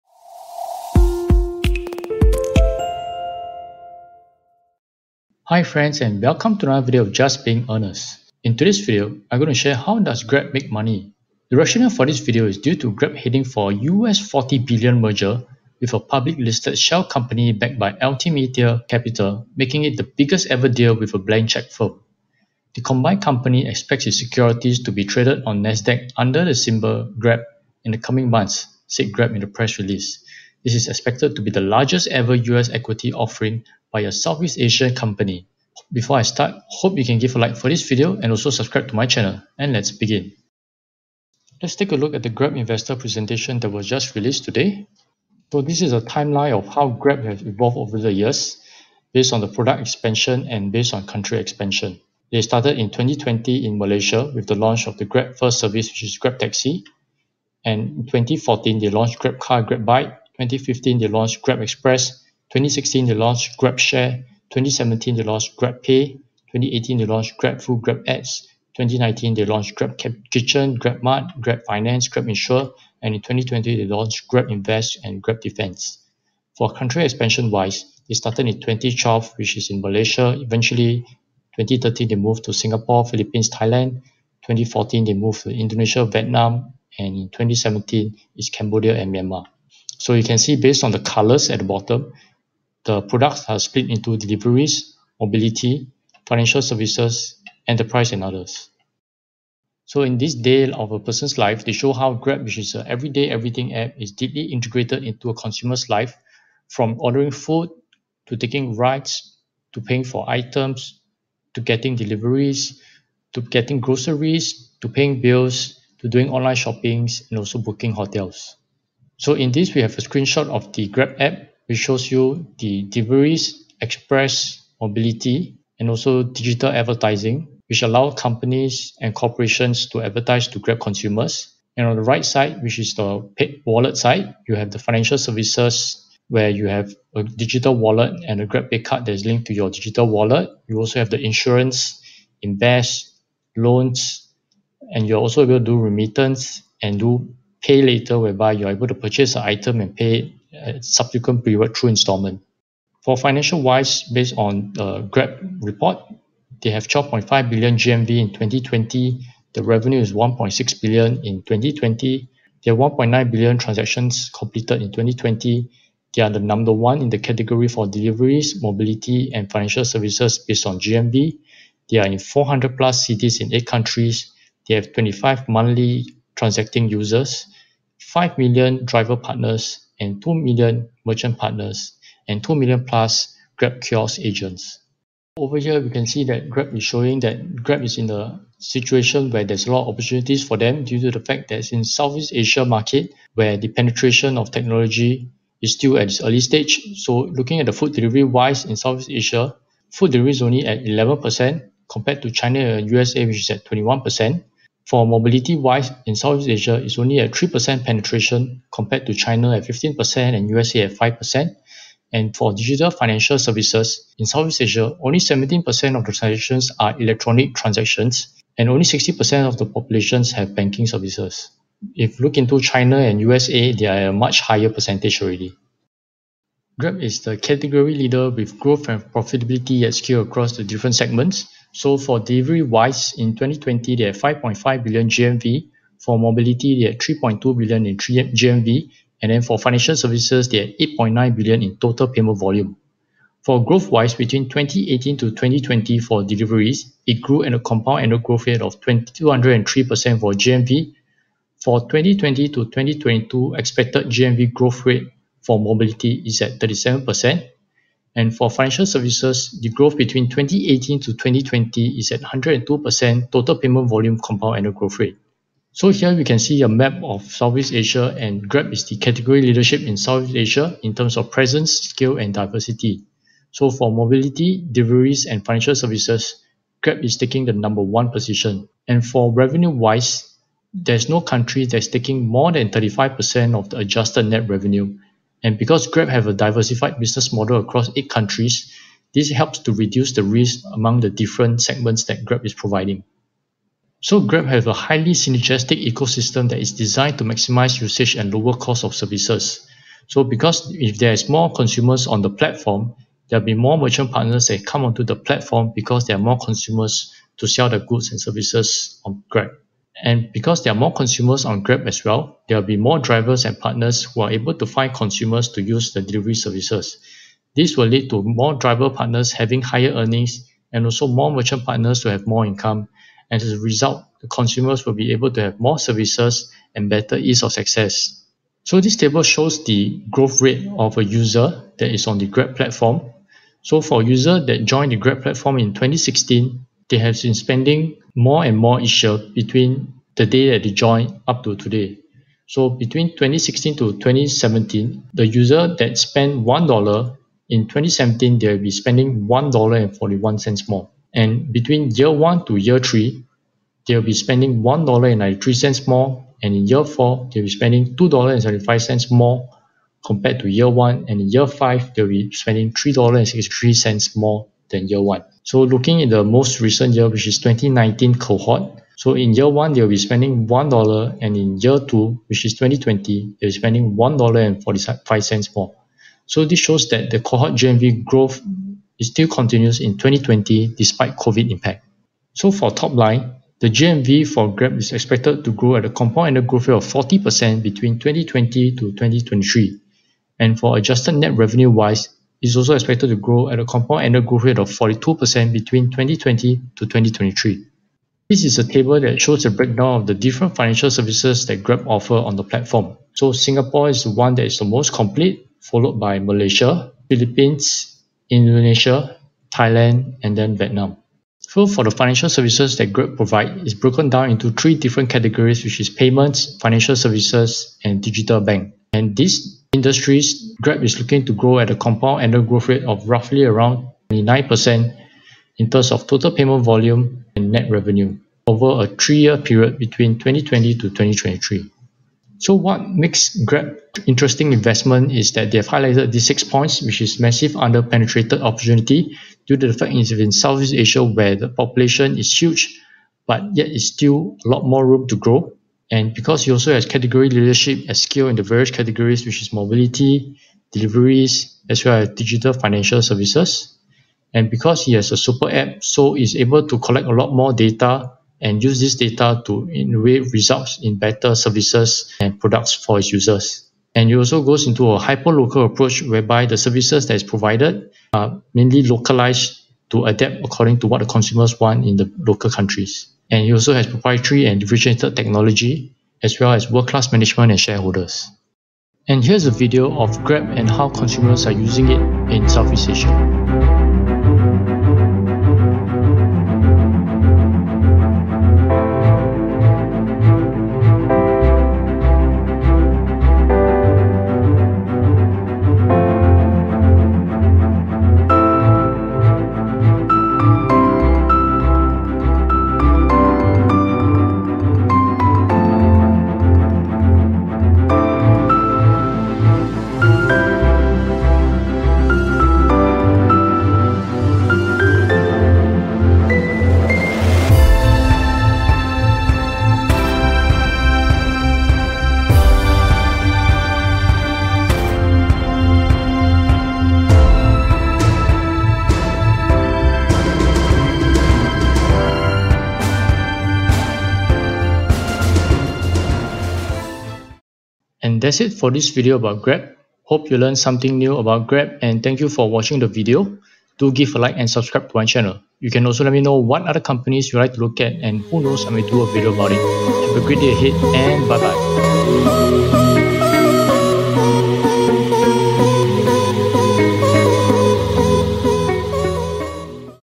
How Does Grab Make Money | COMPANY REVIEWS
Grab Holdings Inc, a super-app from Singapore, officially announced the initial public offering (IPO) plan on the Nasdaq exchange, United States (US). It operates in 8 countries in South East Asia and in more than 400 + cities.  Despite covid in 2020, Grab still managed to see growth in its topline.

With the proceeds, Grab will use the capital "to build the lowest cost and the most efficient delivery network" by investing in maps and other technology as well as "to revolutionize mobile payments, financial services and digital banking.